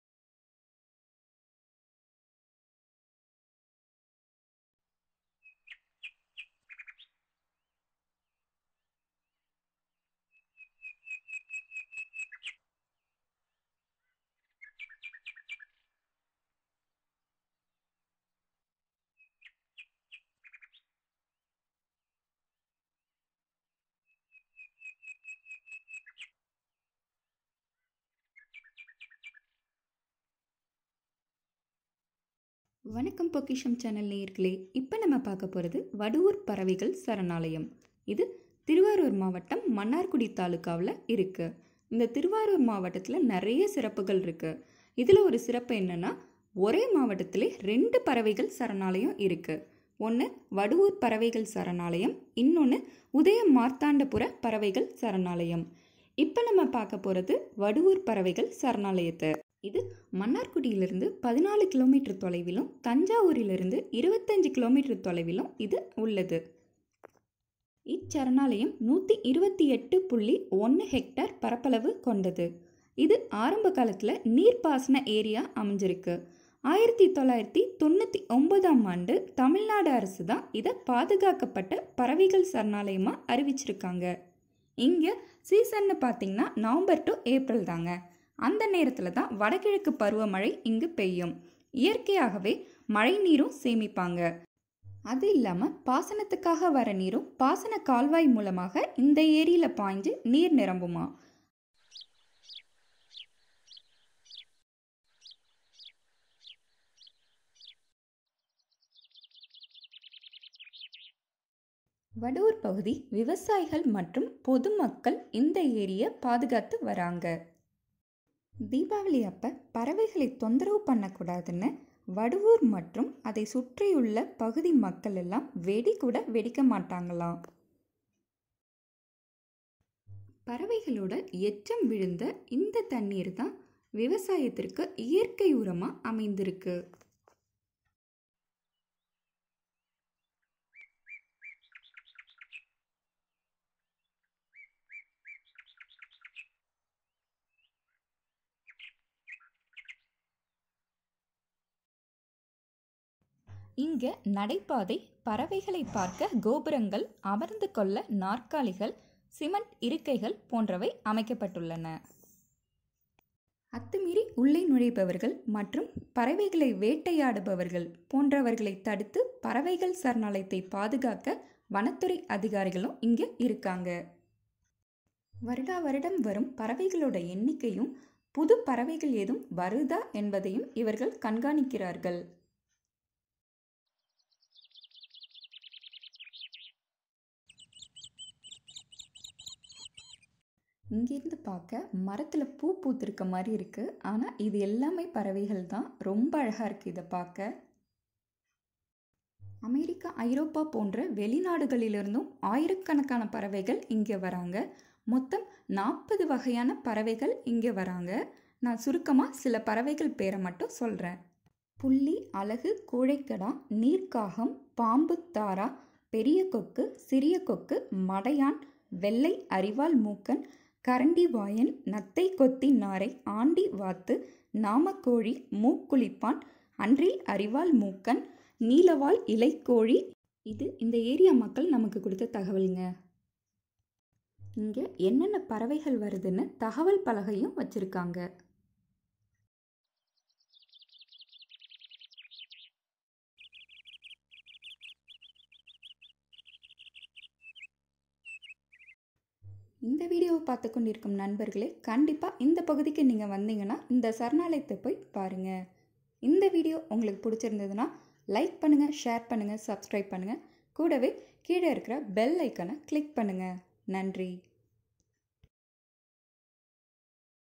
Vaduvoor Birds Sanctuary (வடுவூர் பறவைகள் சரணாலயம்)
Vaduvoor birds sanctuary is located in thiruvarur district between mannargudi and thanjavur . It is situated 14km from mannargudi and 25 km from thanjavur . Birds from as far as america and europe visit the sanctuary from november - april .